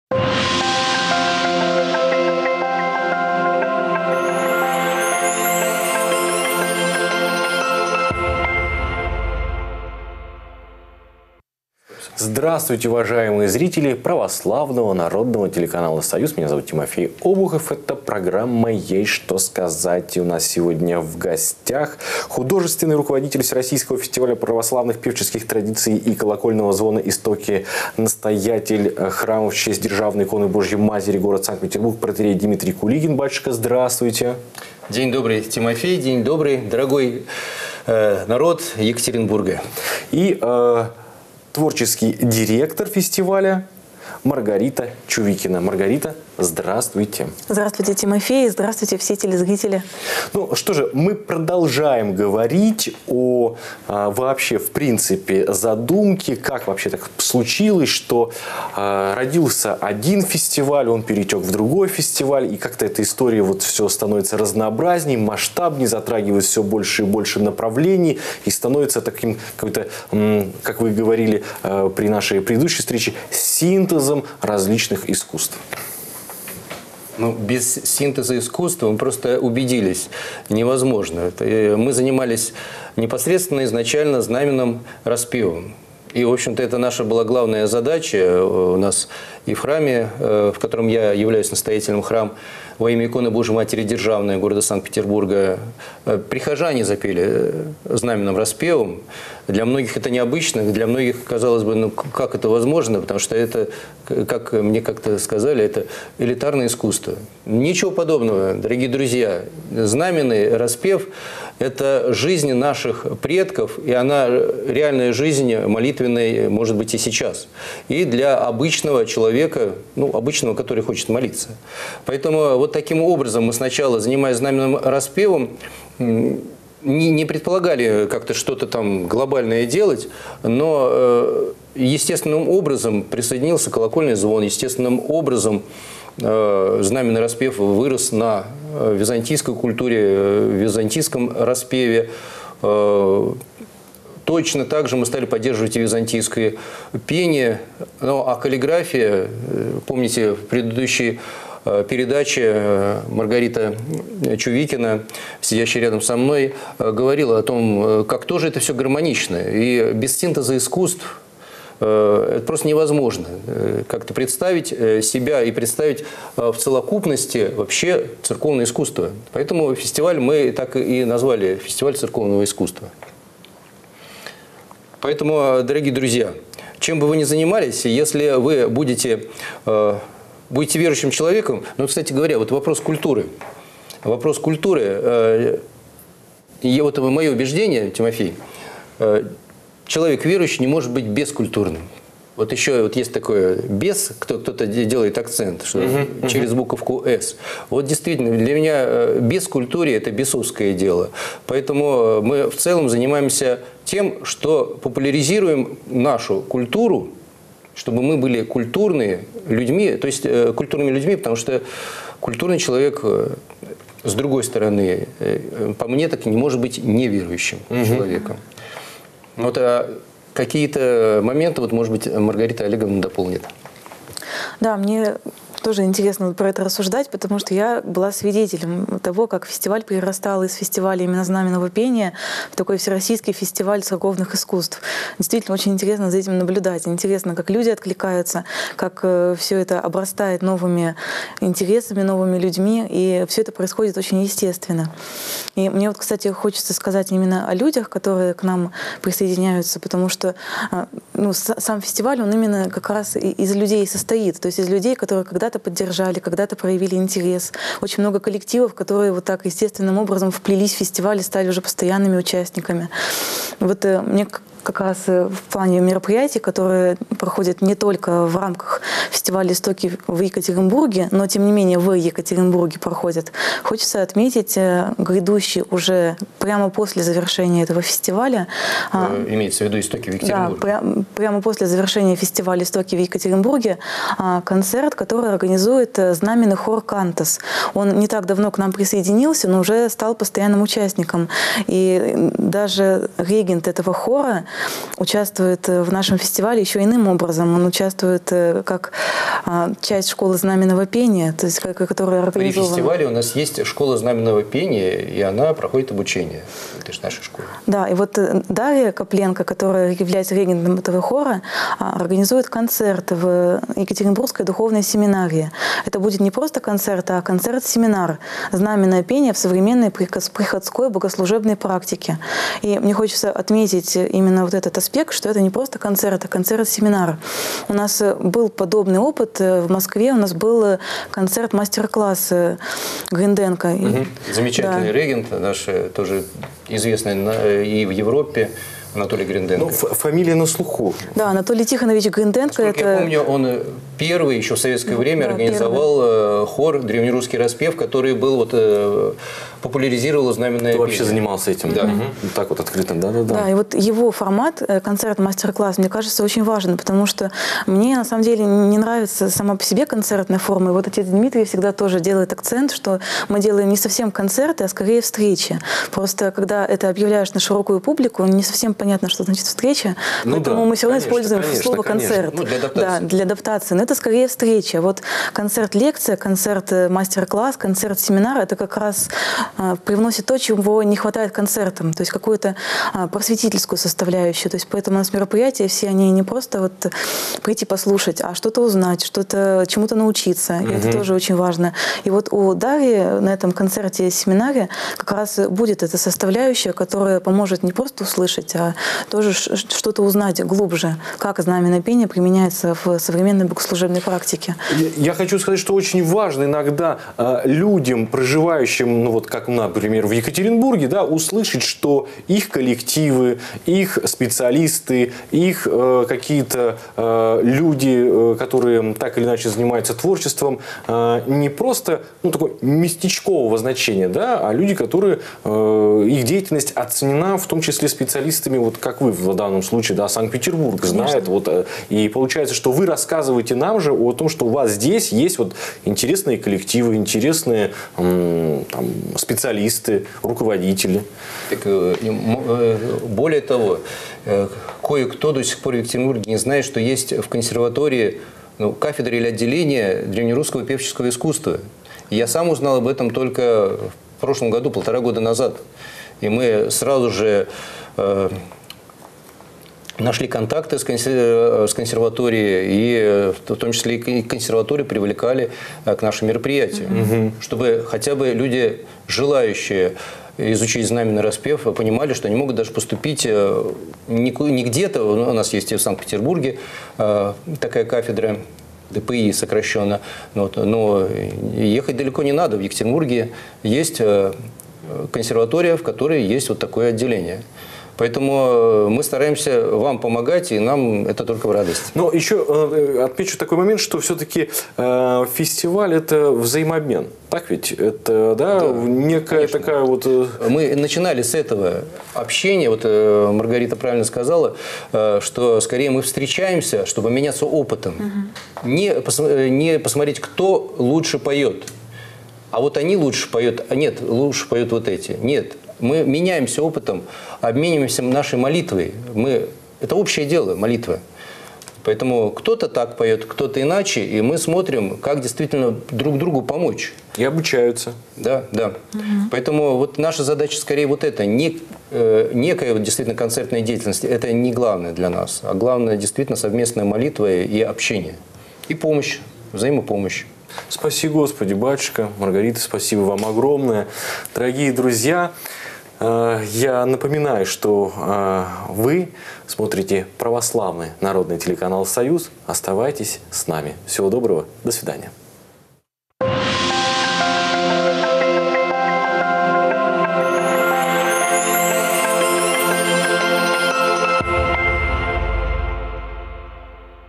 ЕСТЬ ЧТО СКАЗАТЬ: ИСТОКИ-2024 в ЕКАТЕРИНБУРГЕ. 2 часть
С 29 октября по 4 ноября в Екатеринбурге проходит второй фестиваль церковного искусства «Истоки» в Екатеринбурге.

О форуме рассказывают художественный руководитель фестиваля «Истоки» протоиерей Димитрий Кулигин и творческий директор Маргарита Чувикина.